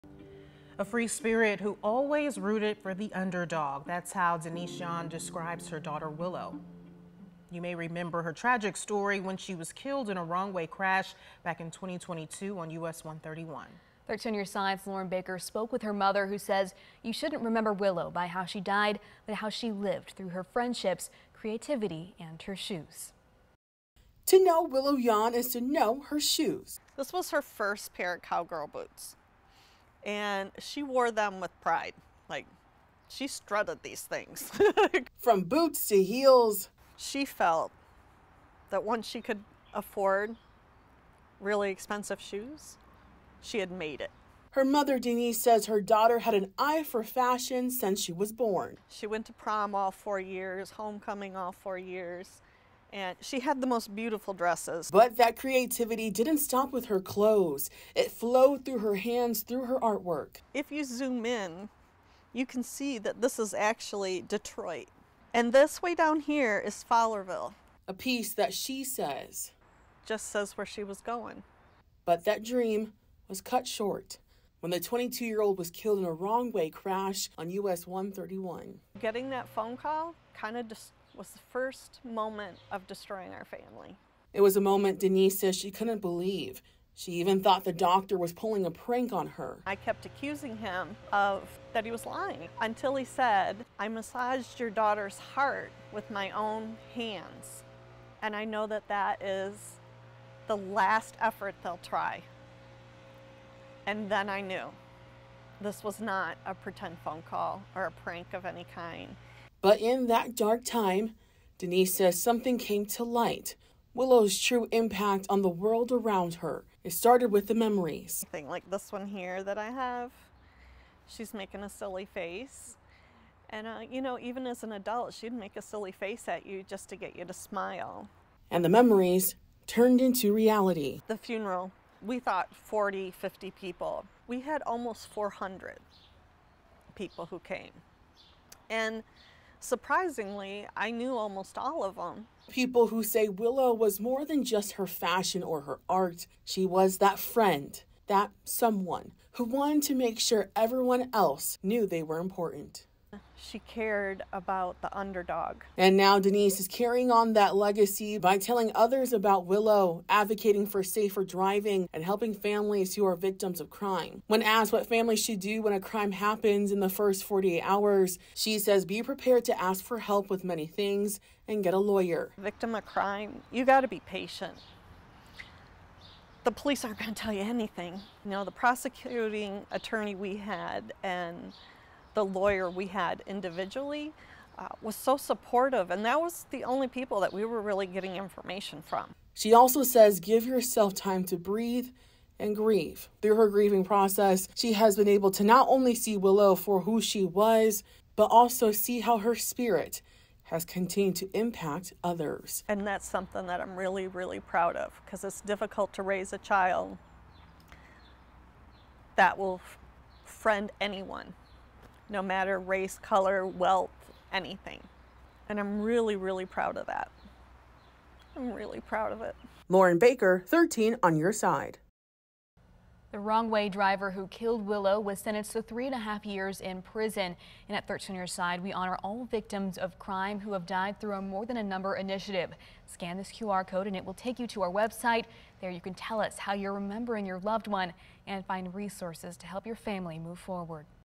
Mother remembers 'free-spirited' daughter who died in 2022 wrong-way crash
Willow Yon’s mother, Denise Yon, said you shouldn’t remember Willow by how she died – but how she lived through her friendships, creativity and her shoes.